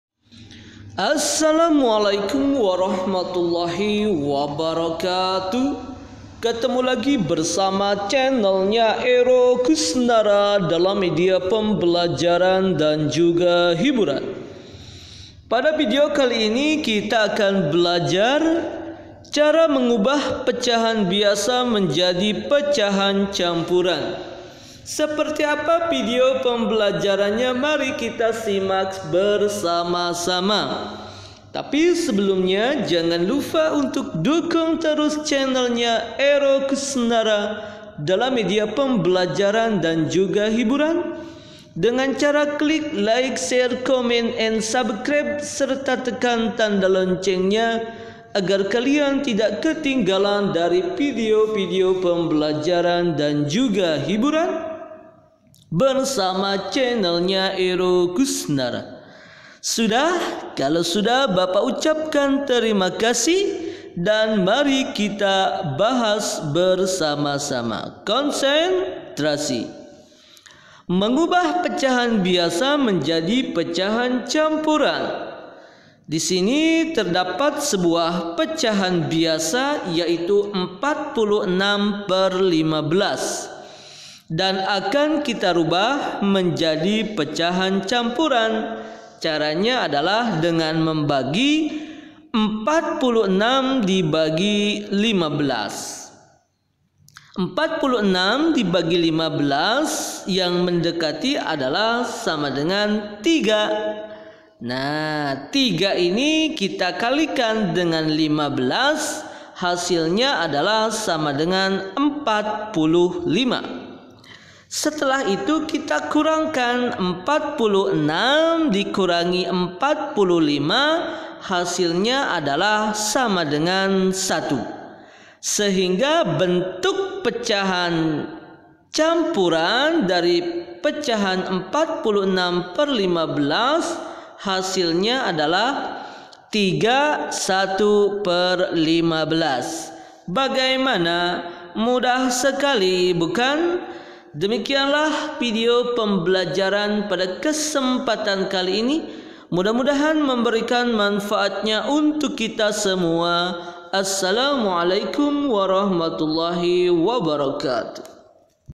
Cara Mengubah Pecahan Biasa Menjadi Pecahan Campuran
Vidio ini berisi media pembelajaran anak sekolah dan hiburan yang edukatif, kreatif dan inovatif.

Pada vidio pembelajaran kali ini kita akan belajar cara mengubah pecahan biasa menjadi pecahan campuran.